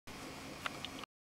Munch chasing Chubby
Munch is half the height of Chubby, but twice the weight, and bullies him a bit...
unfortunately, I missed the beginning of the action, and only caught chubby backing away...